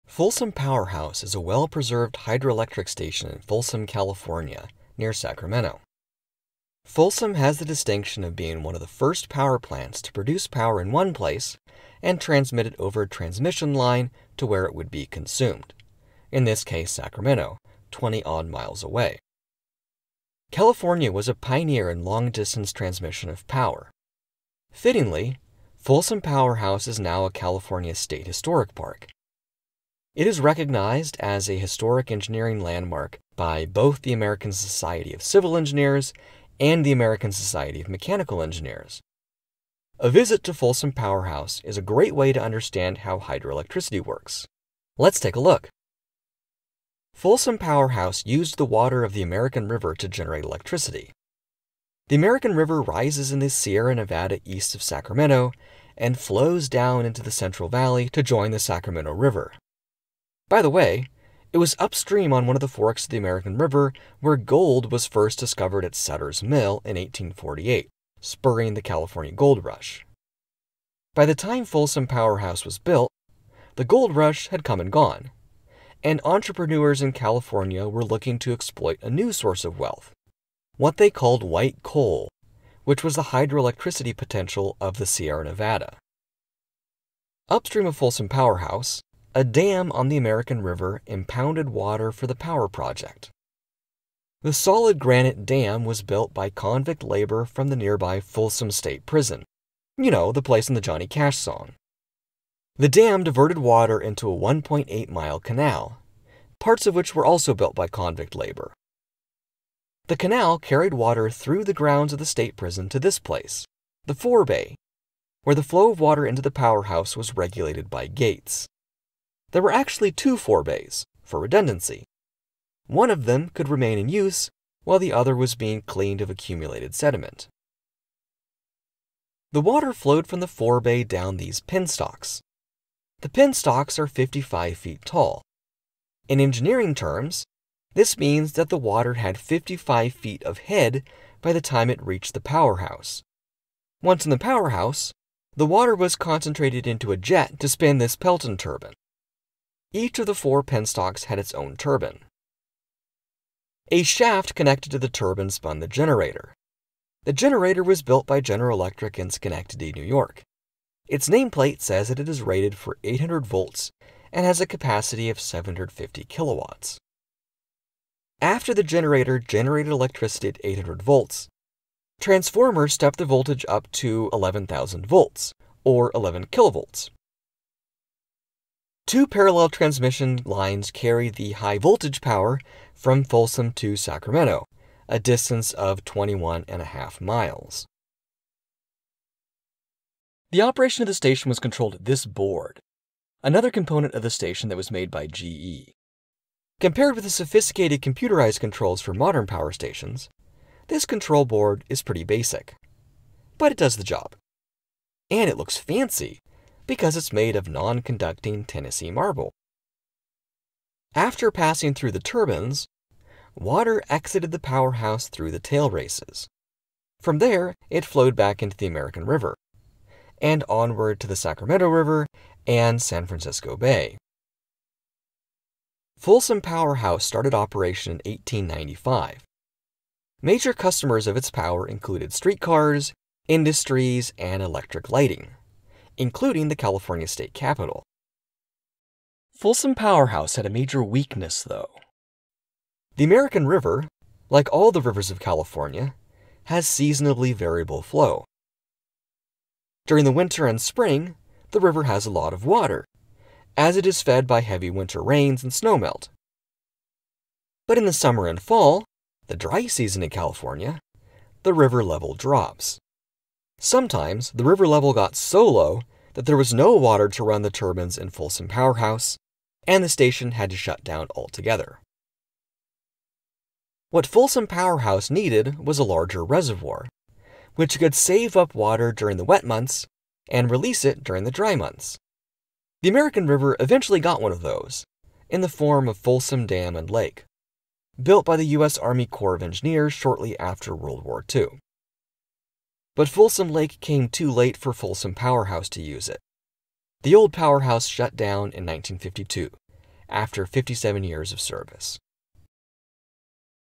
A visit to Folsom Powerhouse
Folsom Powerhouse is a well-preserved hydroelectric station in Folsom, CA. This is a video I made about it for a class I teach about industrialization in the United States. Enjoy!

In case anybody asks, Folsom Powerhouse shut down because it was redundant and in the way of the much larger Folsom Dam (completed 1955).

Archival photos from Wikipedia/Wikimedia Commons, Library of Congress, and HathiTrust.